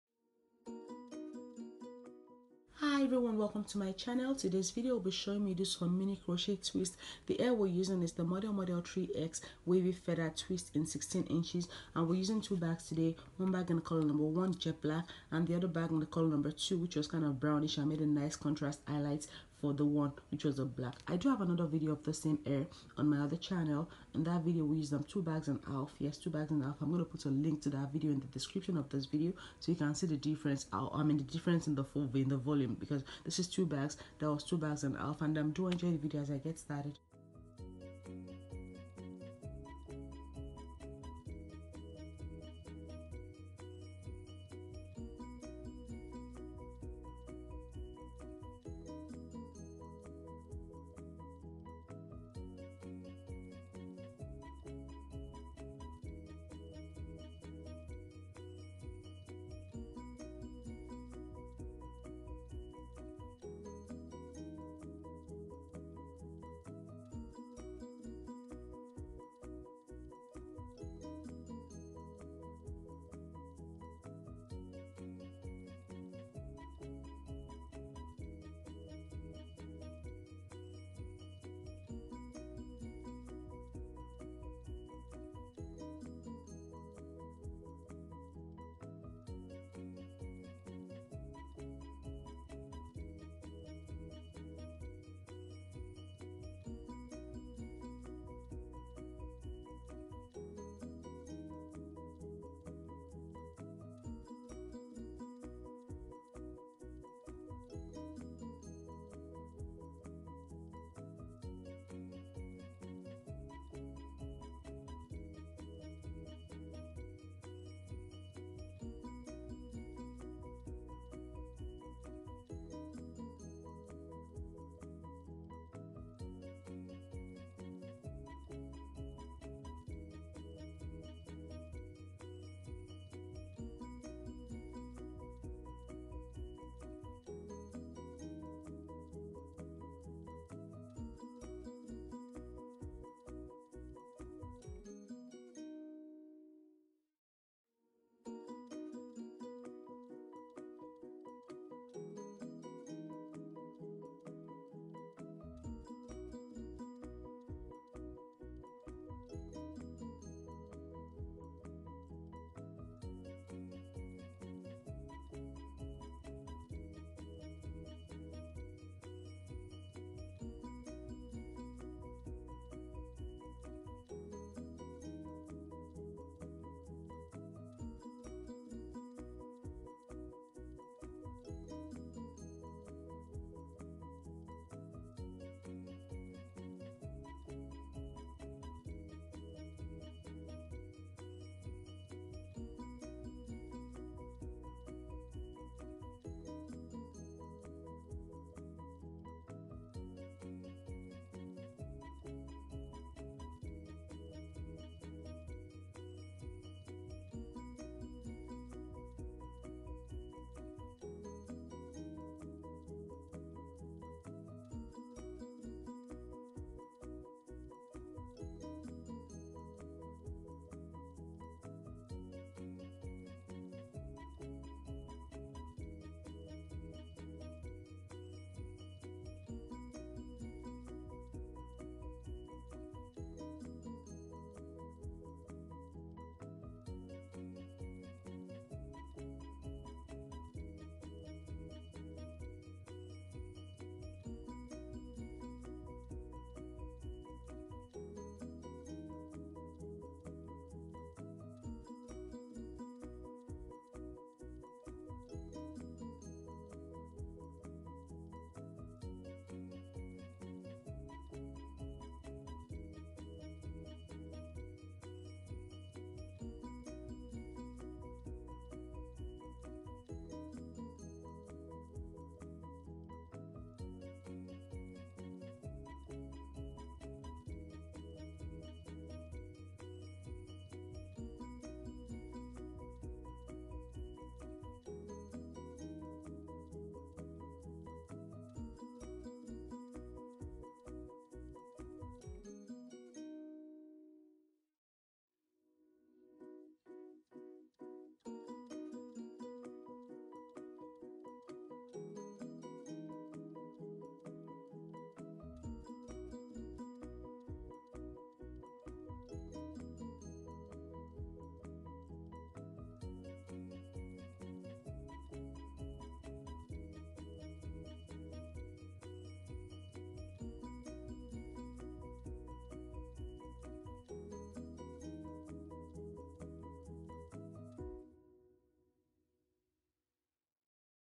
INSTANT TWISTS - WAVY FEATHERED TWISTS 16" CROCHET TUTORIAL
Quick video showing crochet mini twists done.